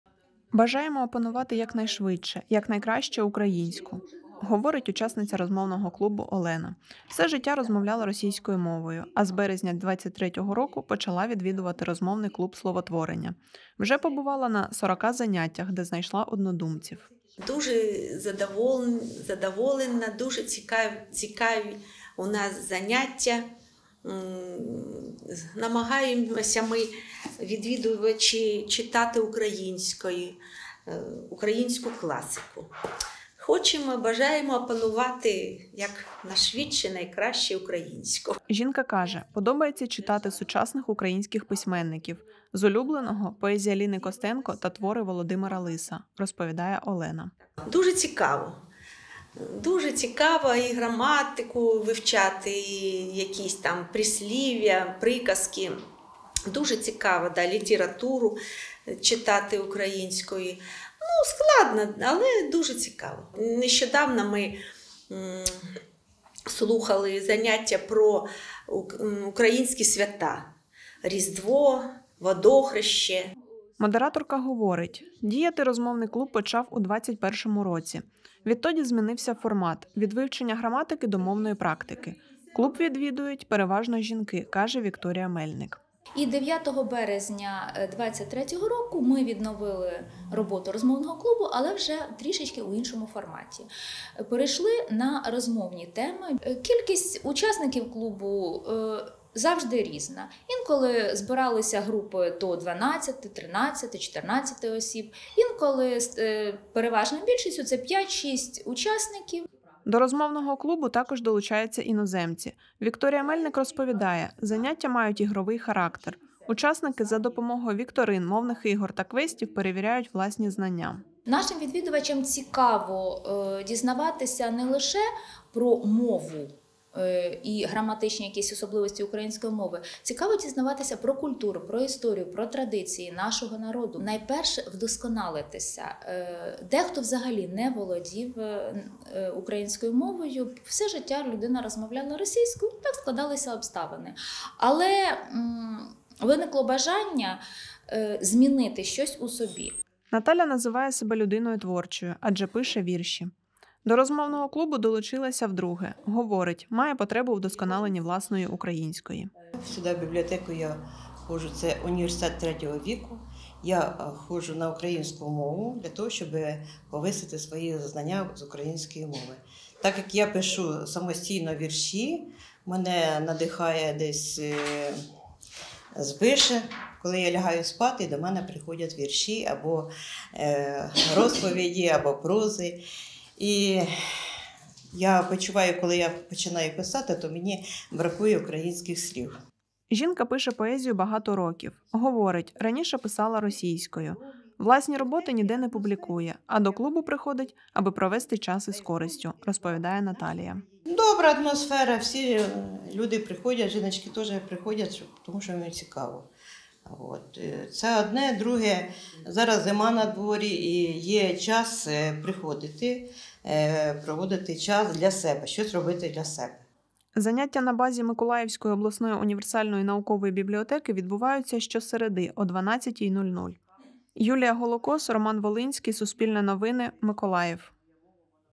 На базі бібліотеки в Миколаєві діє розмовний клуб
У Миколаєві на базі обласної універсальної наукової бібліотеки з 2021 року функціонує розмовний клуб "Словотворення". Охочі можуть вивчати та вдосконалювати українську мову в ігровому форматі.